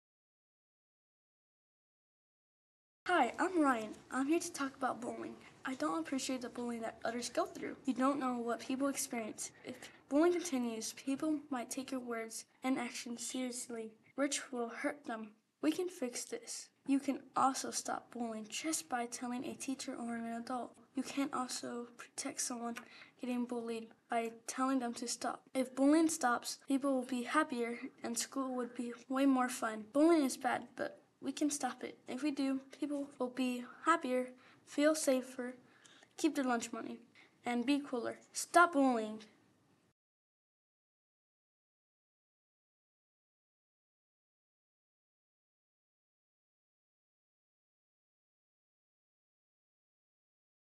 No excuses for animal abuse PSA - Summer Animation Camp Project
Lytle ISD is always looking for ways to motivate our students to practice writing and to learn technology skills for their future. This summer, we piloted a program that helps Elementary students to learn animation skills to bring their writing to life. The program involved students who volunteered to participate in Animation Summer Camp, as well as our students who were invited to the Jolly Roger Summer Academy. The verdict was overwhelming - students 100% LOVED the program. Watch for yourself. You'll enjoy their writing, performances and their creative animations. Special thanks to their teacher, Mr. Pedro Rodriguez and five Lytle High School Skull Studios students who worked as tutors leading these young animators to success. We couldn't do it without our partnership with all the nice folks at Wonder Media who are curriculum and animation experts.